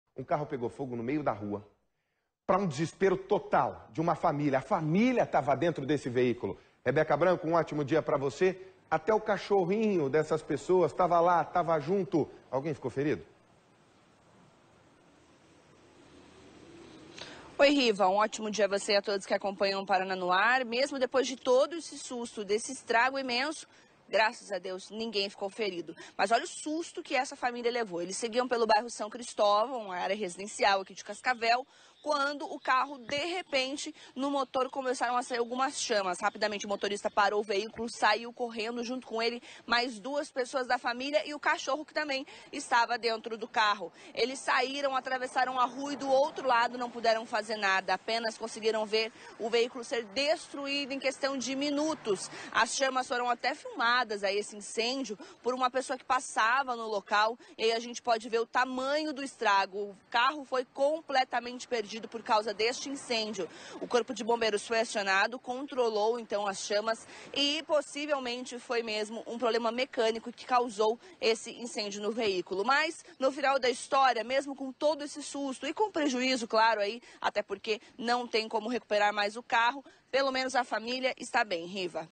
Carro pega fogo no meio da rua com família dentro em Cascavel
A família seguia com o carro pelo bairro São Cristóvão, em Cascavel, quando o motor começou a pegar fogo. Todos conseguiram sair do veículo e ninguém ficou ferido.